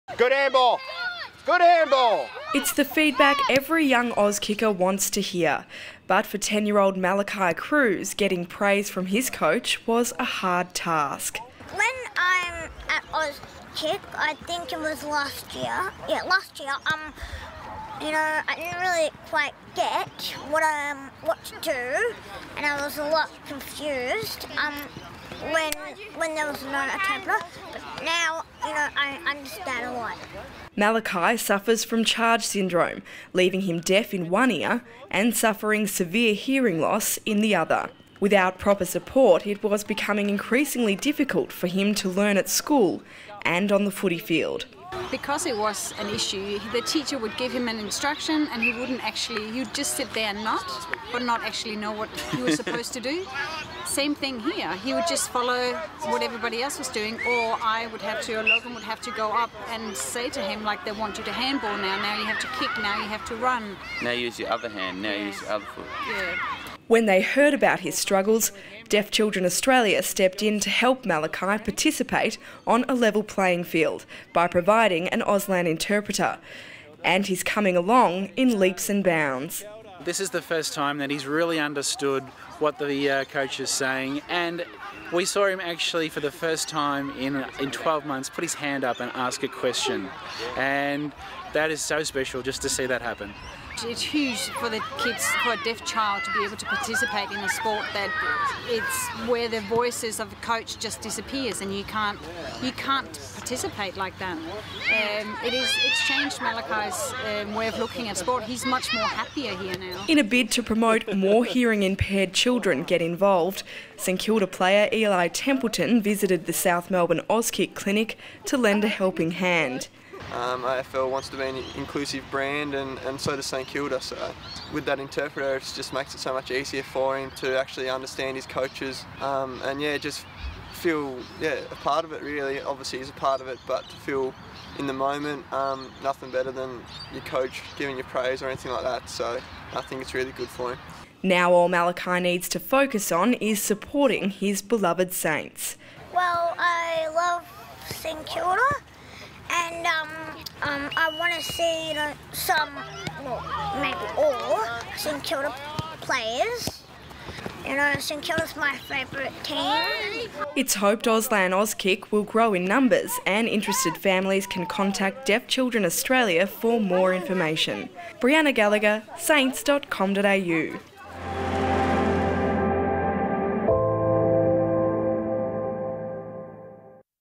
Eli signs young Saint
Eli Templeton lent a helping hand at an Auslan Auskick clinic for young Saints fan, Malachi Kruse.